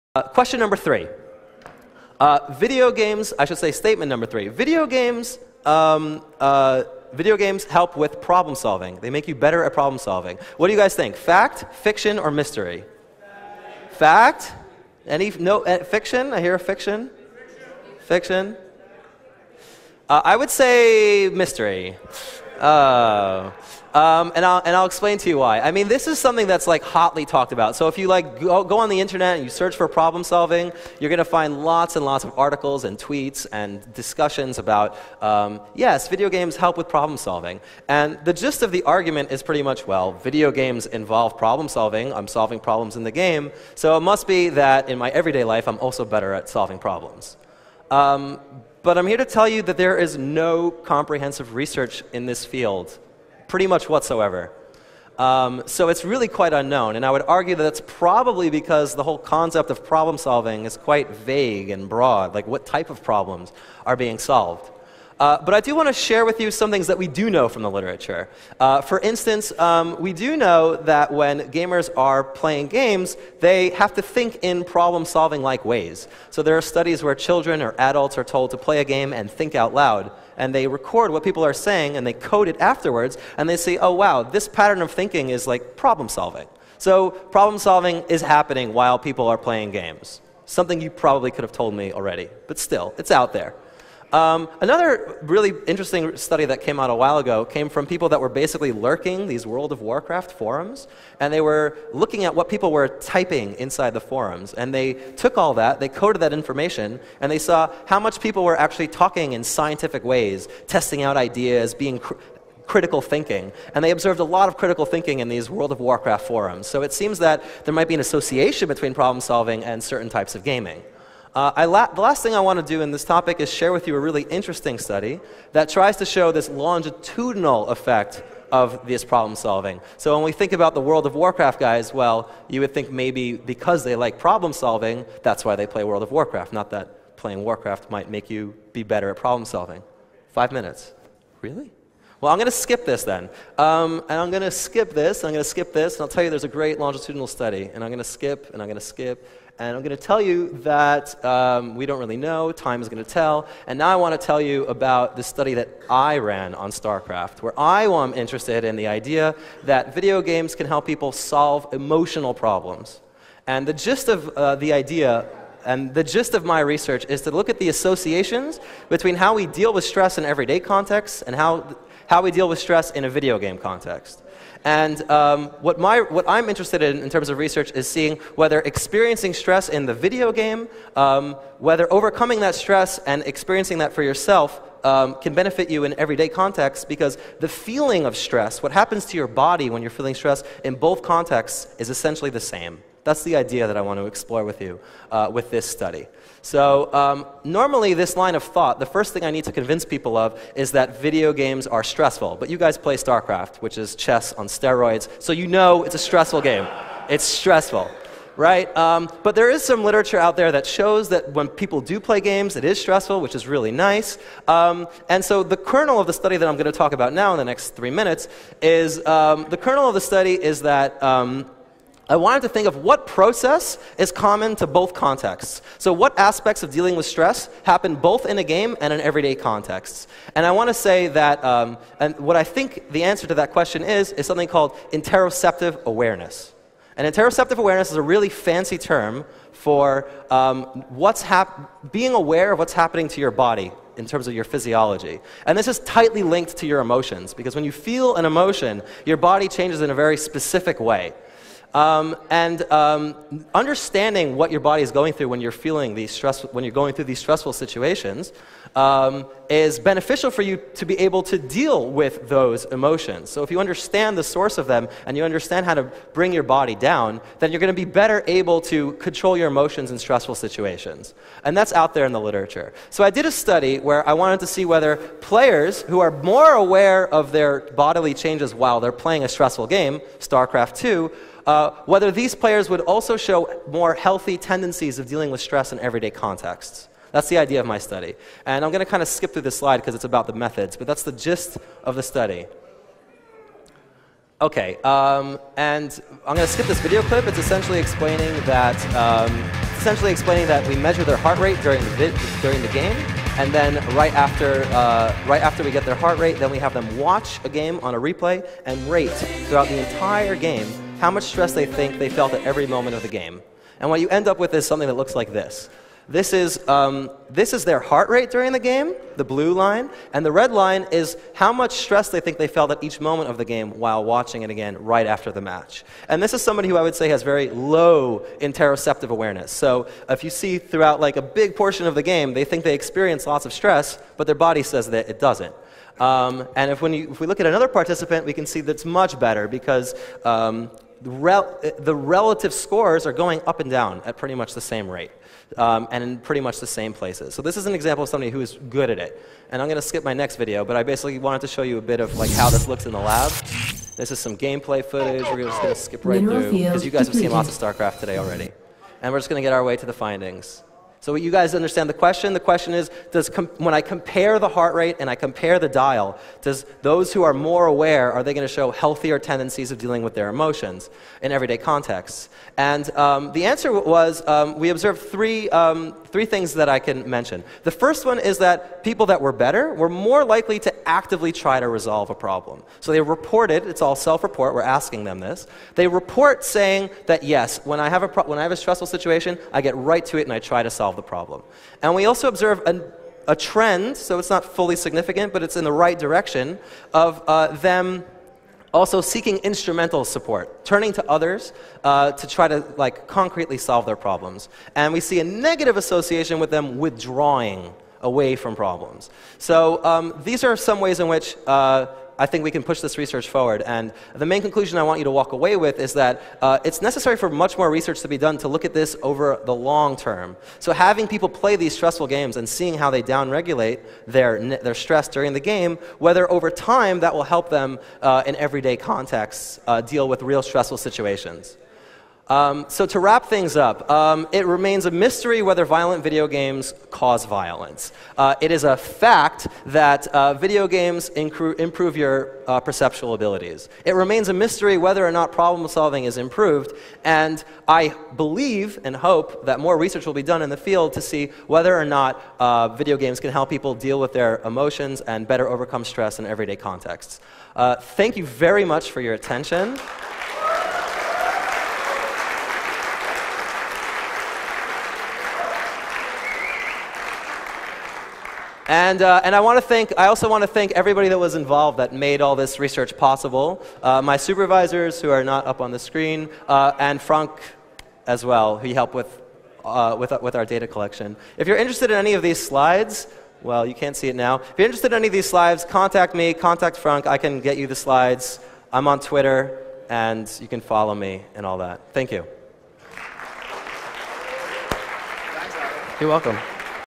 Video Games and Psychology: Fact, Fiction, and Mystery 2/2
In which I discuss what Psychology has to say about the effects of playing video games and why I think stressful games can be of everyday benefit. I still use funny hand gestures too.

This talk was given at the Dutch Starcraft League Finals (2013). Check them out